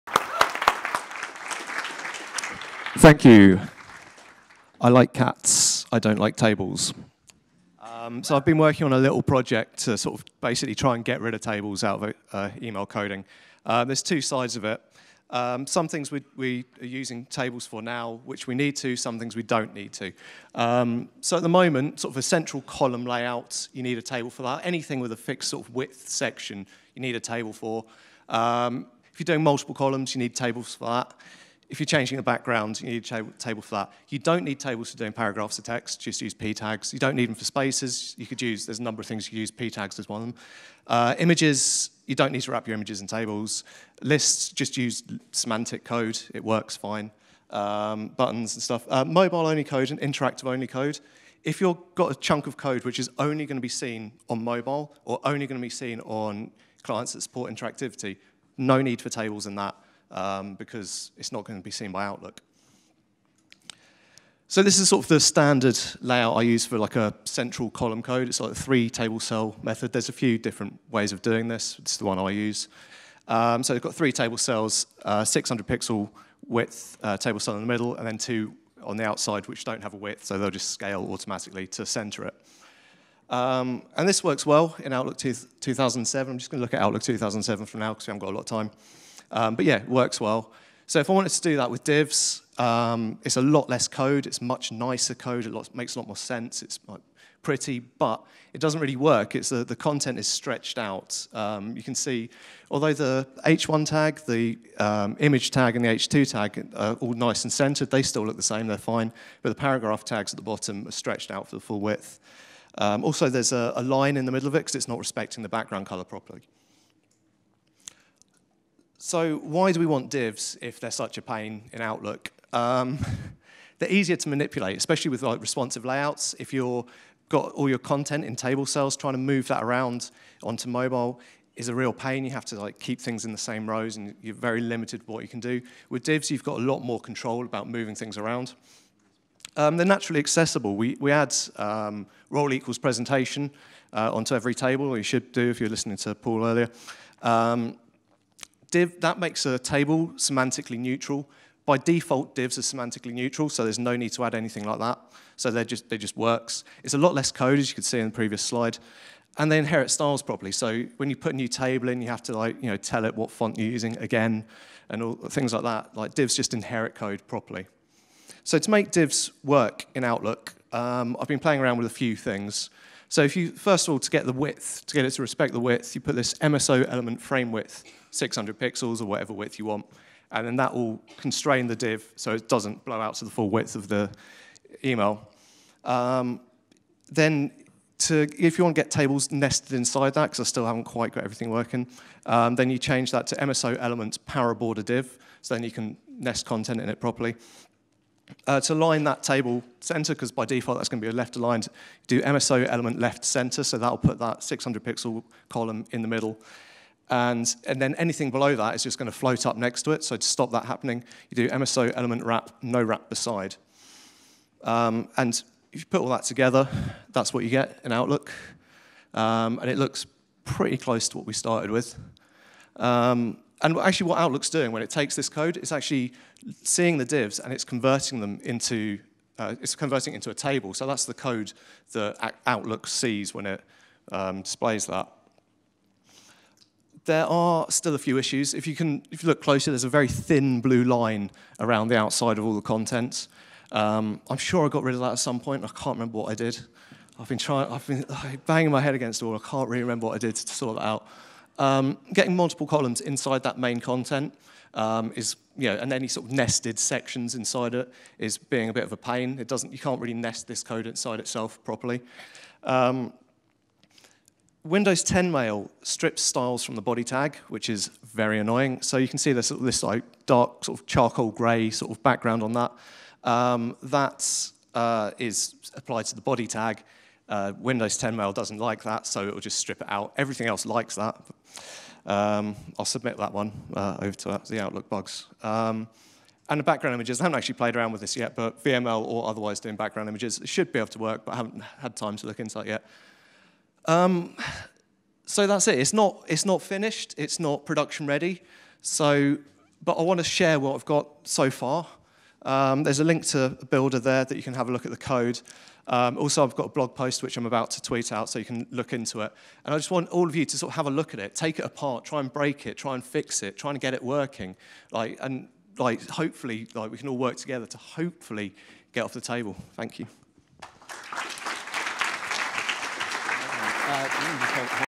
Get Off the table by Mark Robbins
Watch and learn how to use just divs to build an email, rather than having to rely on tables.